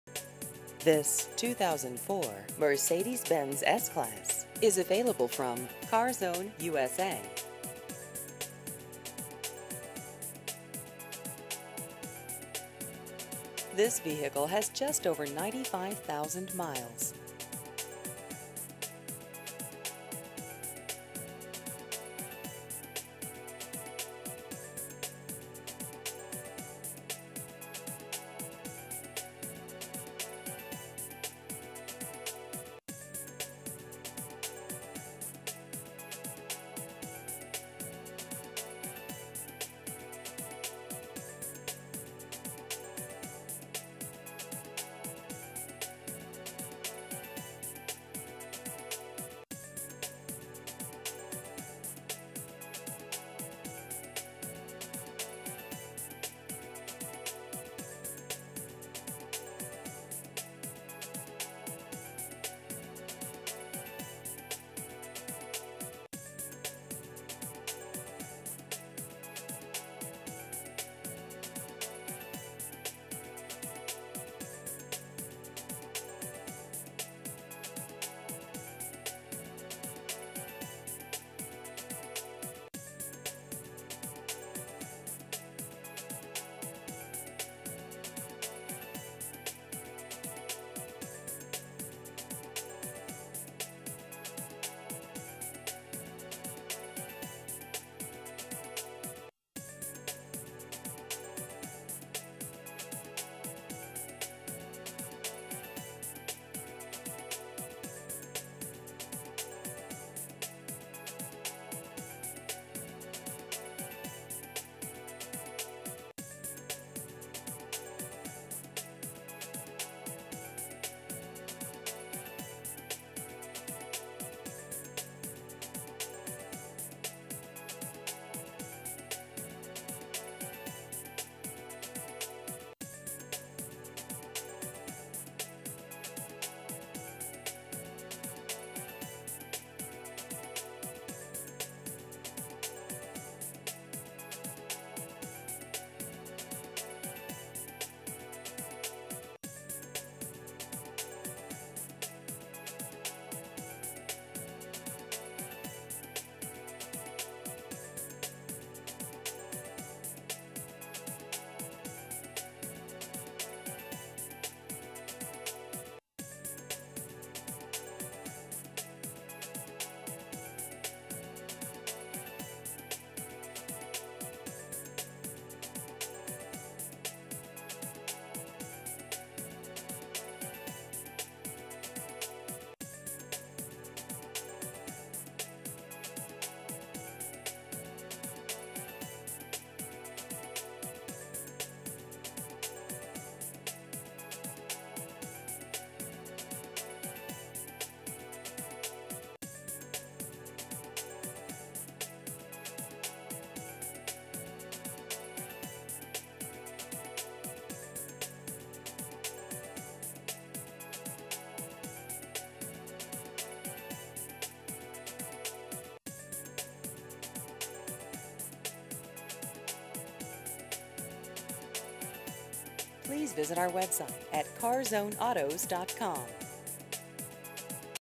2004 Mercedes-Benz S-Class Used Cars Baltimore Maryland
This 2004 Mercedes-Benz S-Class is available from CarZone USA.

Year: 2004
Make: Mercedes-Benz
Model: S-Class
Trim: S430 4MATIC with NAV and DVD and Parking Aid
Mileage: 95,224
Trans: Automatic
Vin: WDBNG83J04A390150
Stock #: 390150
Engine: 4.3L V8 SOHC 24V
Interior: Leather
Color: Horizon Blue Metallic
Drive Train: AWD
Type: Sedan
State: MD